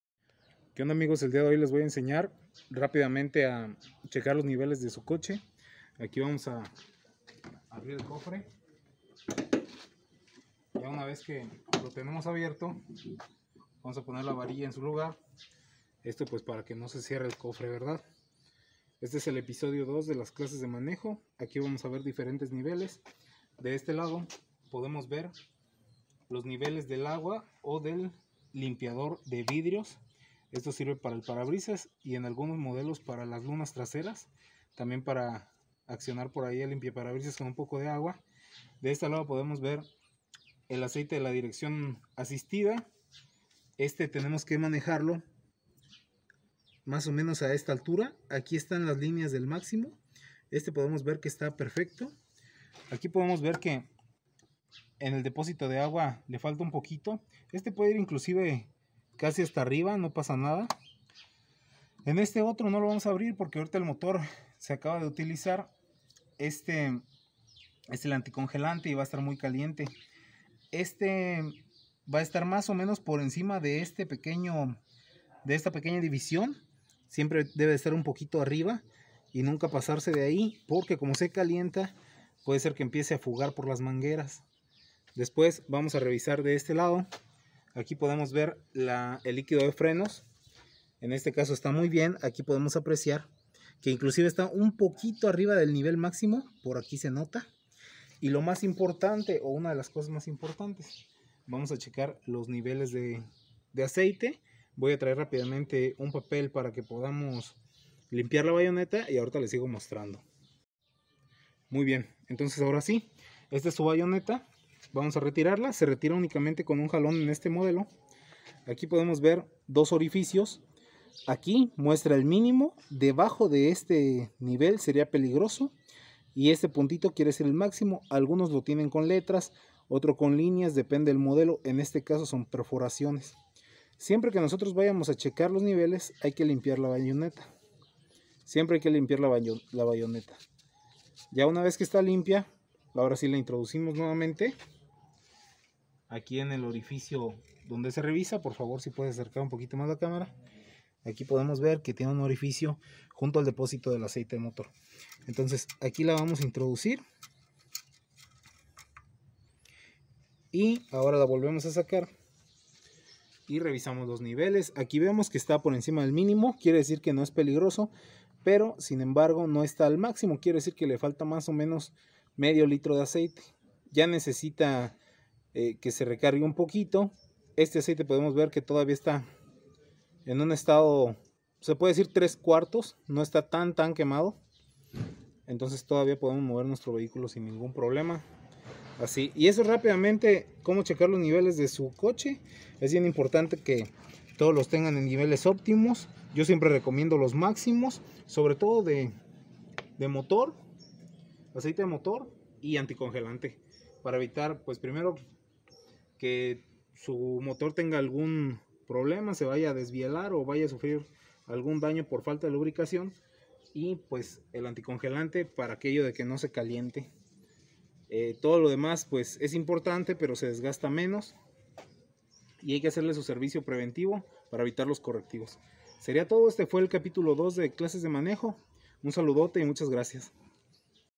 cómo CHECAR NIVELES de un auto 🚙💧 clases de manejo desde CERO | episodio # 2 (aprende YA)
🚗🤩 Necesitas aprender a revisar los niveles antes de manejar!!! ( aprende en este video.)